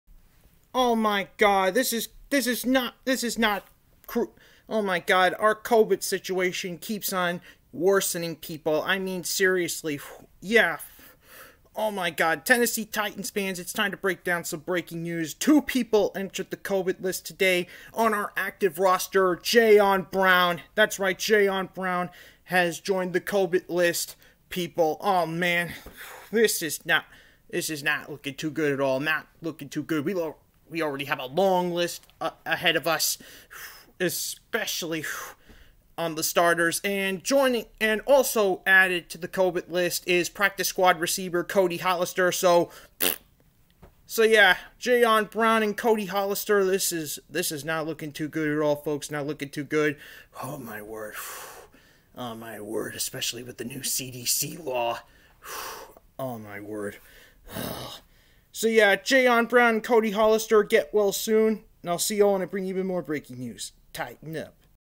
JAYON BROWN AND CODY HOLLISTER BECAME TITANS LATEST COVID VICTIMS!!
Breaking News: Tennessee Titans has placed Linebacker Jayon Brown and Practice Squad Wide Receiver Cody Hollister on the COVID-19 list!!